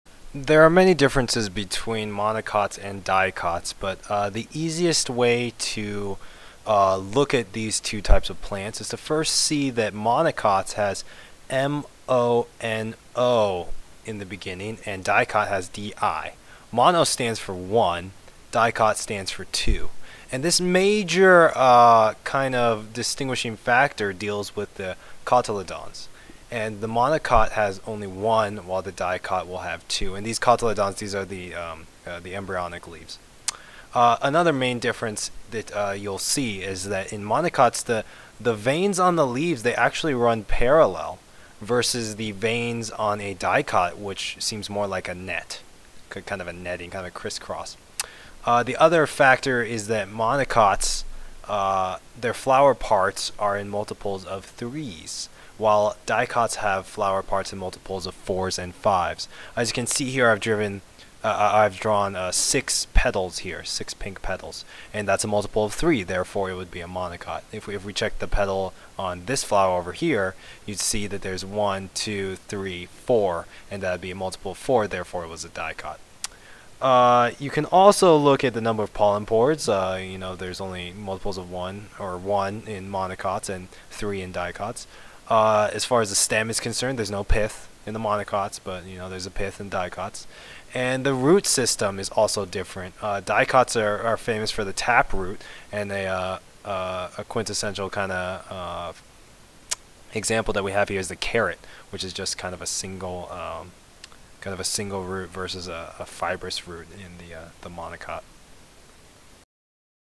Biology Monocots vs Dicots
Discussing the differences between monocot plants and dicot angiosperms.